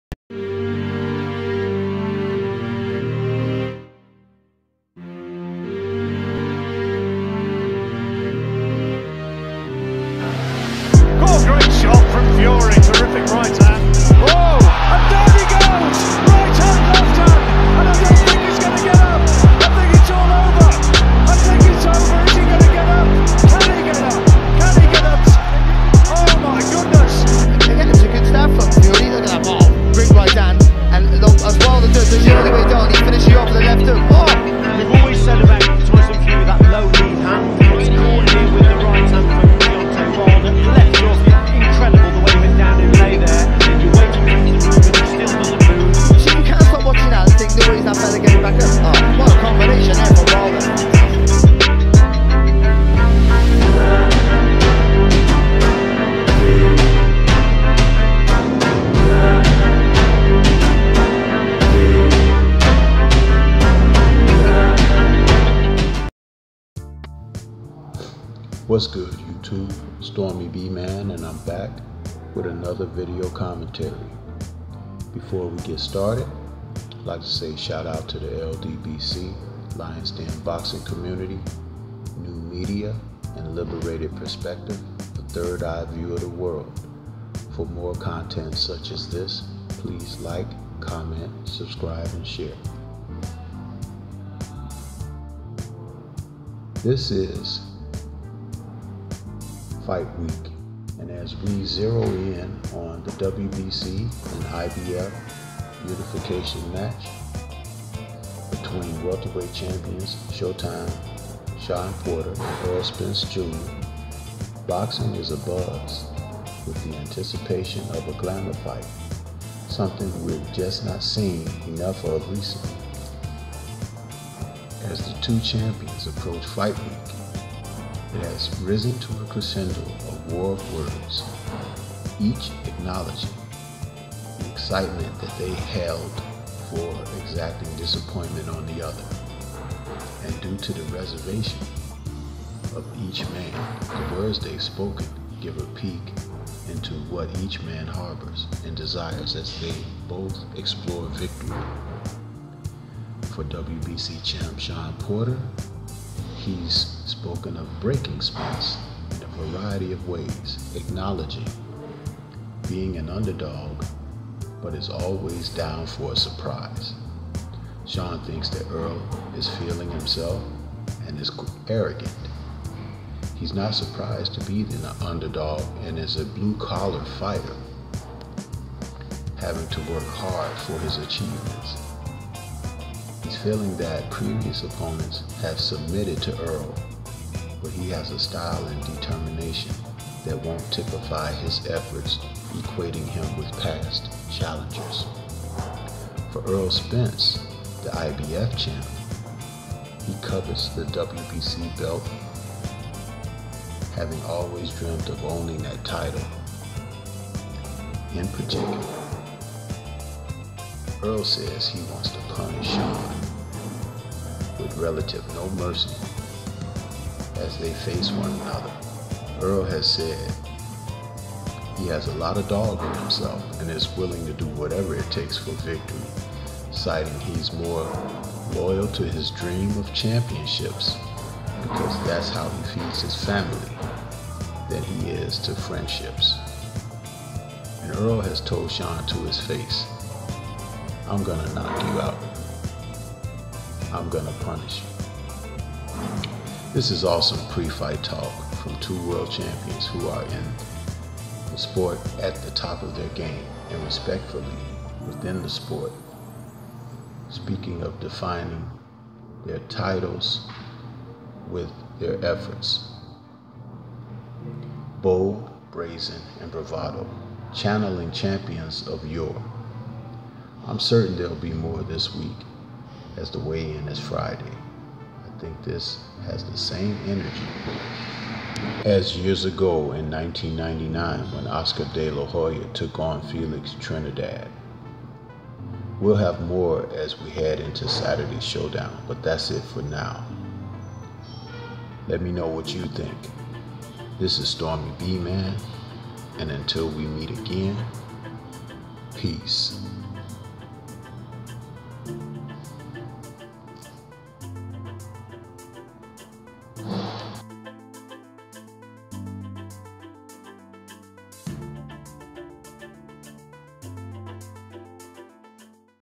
STORMYBMAN: THE WBC/IBF WELTERWEIGHT UNIFICATION *IT'S FIGHT WEEK
Boxing: Talk, Commentary, Editorial
*WARNING: Graphic and Adult Language